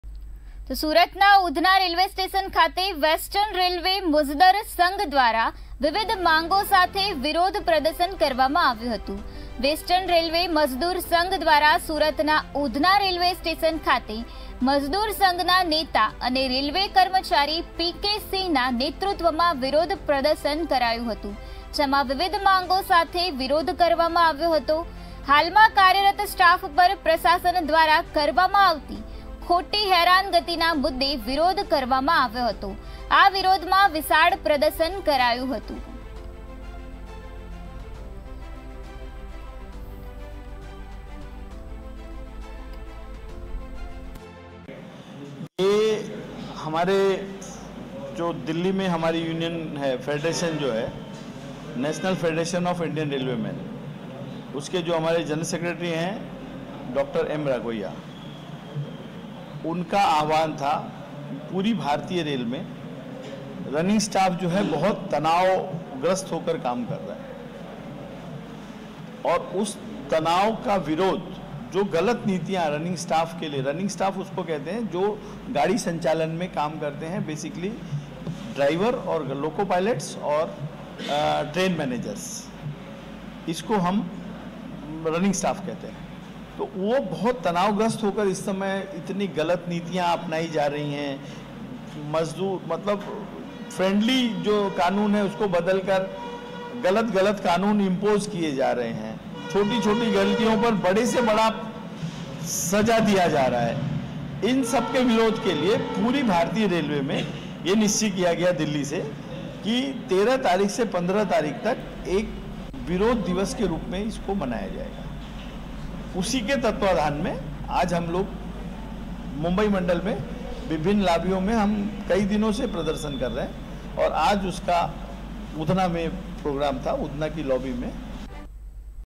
સુરતના ઉધના રેલ્વે સ્ટેશન ખાતે વેસ્ટર્ન રેલ્વે મઝદુર સંઘ દ્વારા વિરોધ પ્રદર્શન
SURAT: ઉધના રેલ્વે સ્ટેશન ખાતે વિરોધ પ્રદર્શન
રેલ્વે મઝદુર સંઘ દ્વારા વિરોધ પ્રદર્શન કરાયું
વિવિધ માંગો સાથે વિરોધ નોંધાવ્યો